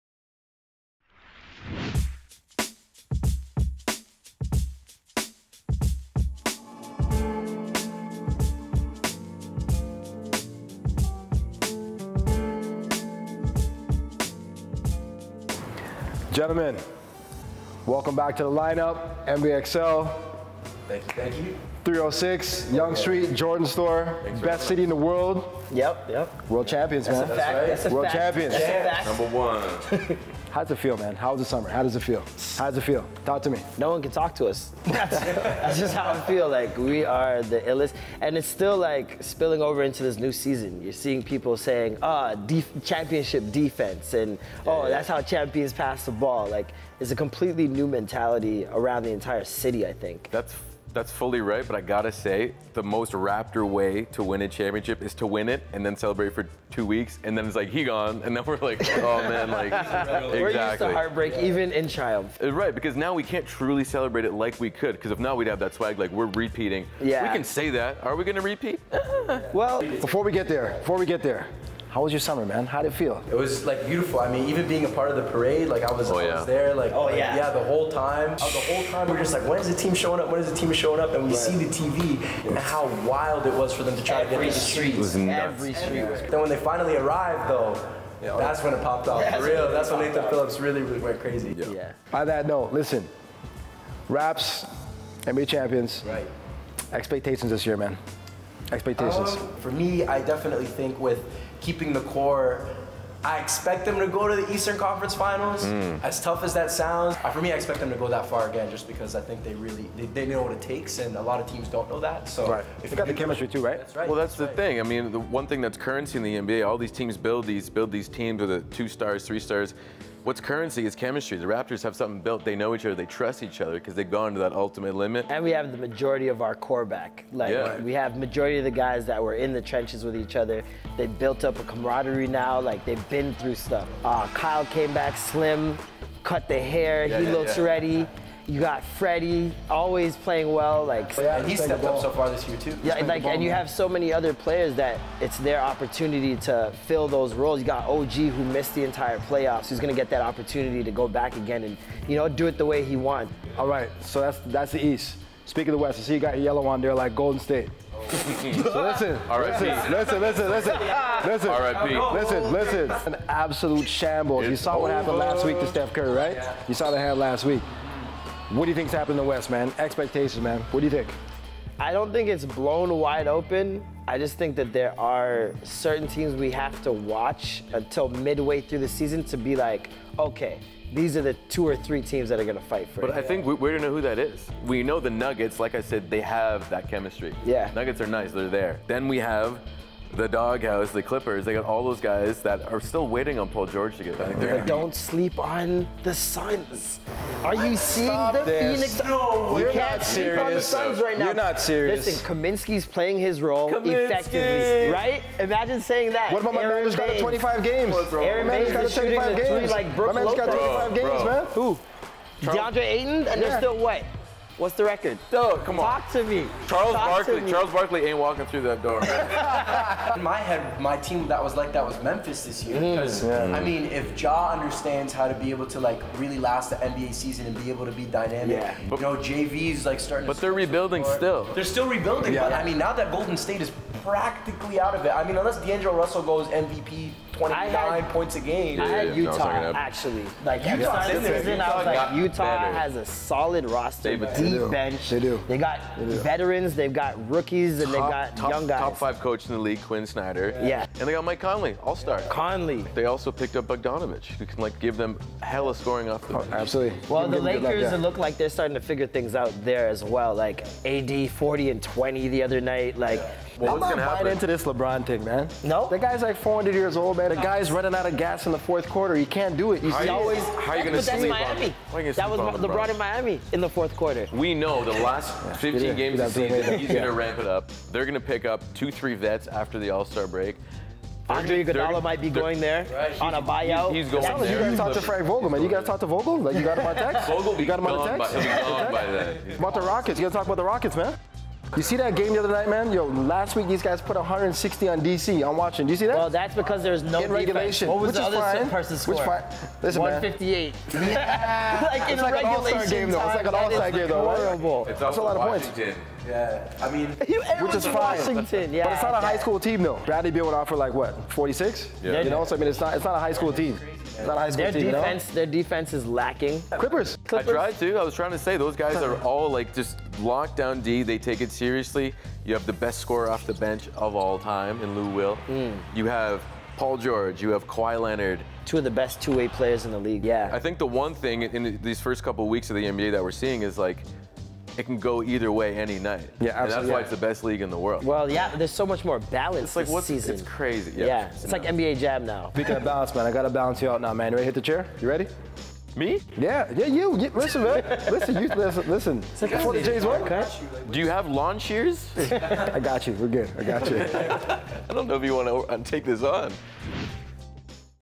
Toronto Raptors: New Season, Who Dis? I NBA XL
Patrice and the Line Up crew reflect on the Toronto Raptors championship run and their expectations for this season. They also discuss their Western Conference favourites and which teams may make a jump.